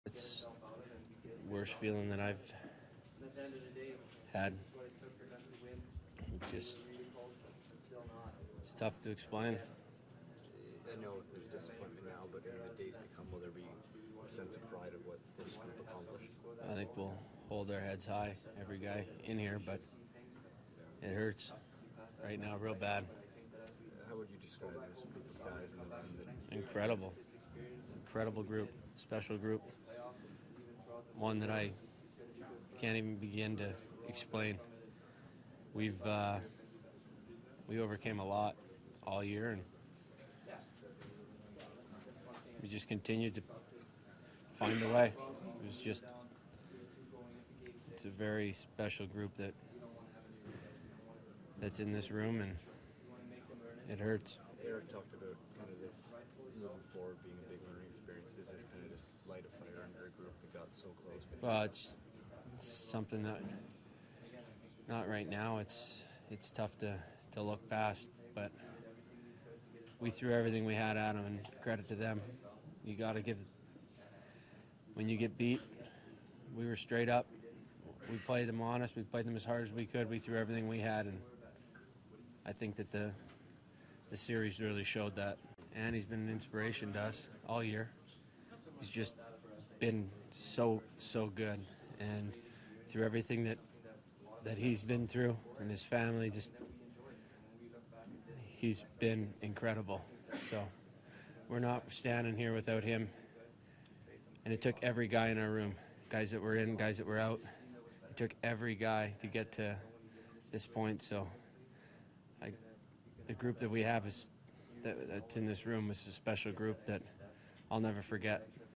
Phaneuf: Worst feeling I’ve had, it’s tough to explain
An emotional Dion Phaneuf spoke post game about the Senators Game Seven loss, how special the group of teammates he has, and how inspirational Craig Anderson has been this season.